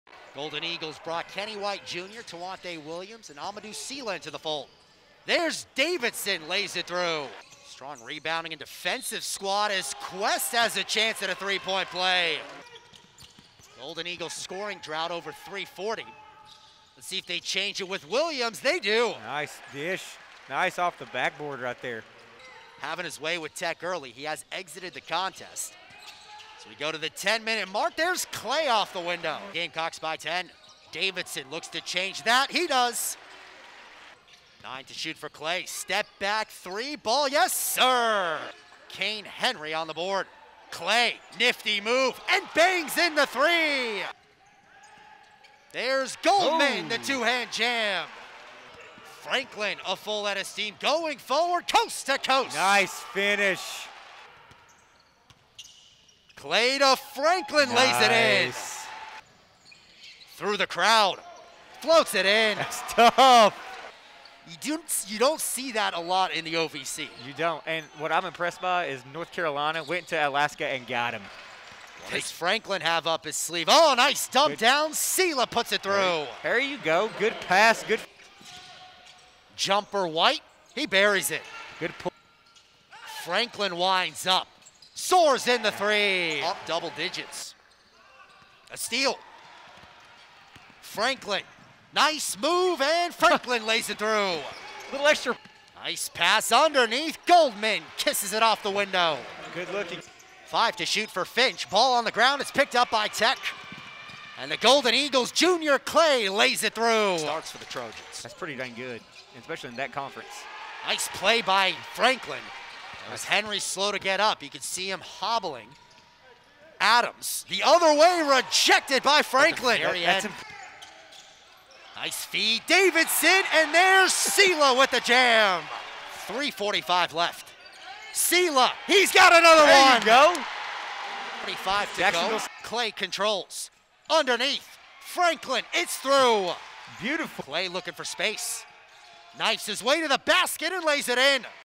Highlights-Men's Basketball vs Jacksonville State 12/13/20
Final:
Jacksonville State-73
Tennessee Tech-67